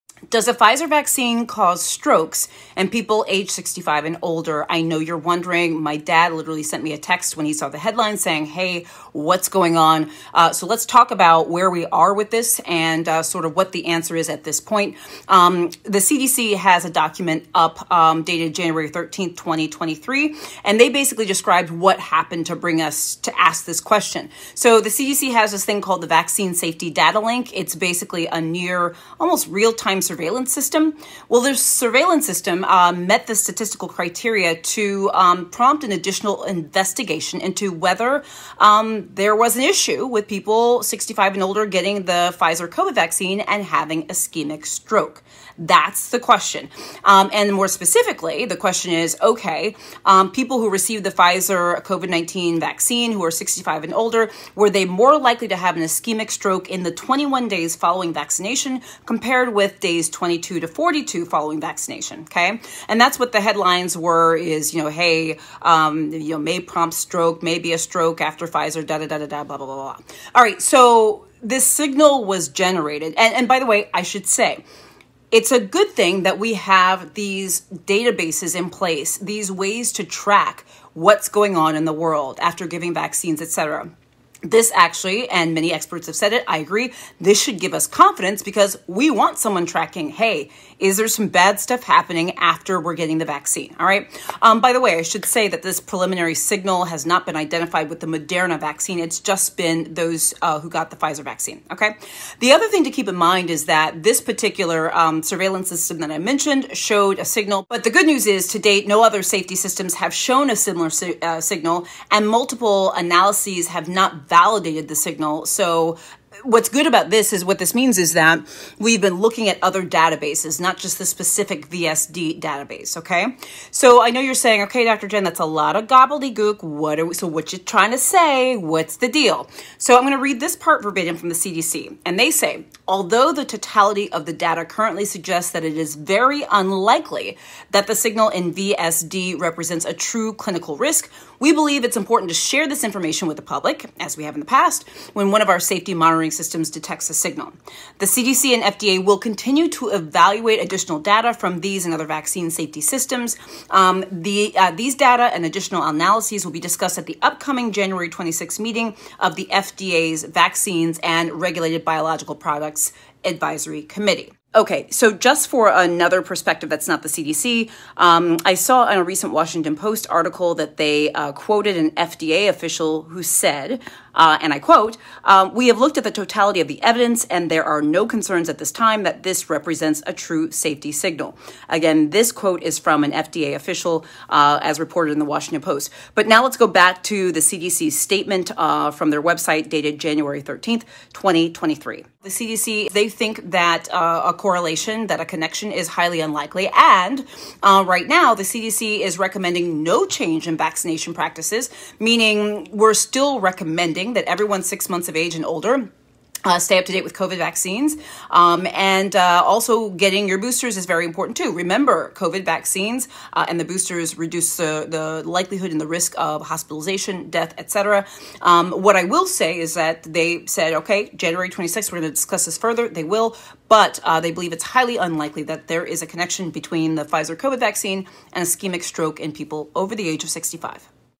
Does the Pfizer COVID Vaccine Cause Strokes in People Over 65? A Doctor Discusses
Does the Pfizer COVID Vaccine Cause Strokes in People Over 65?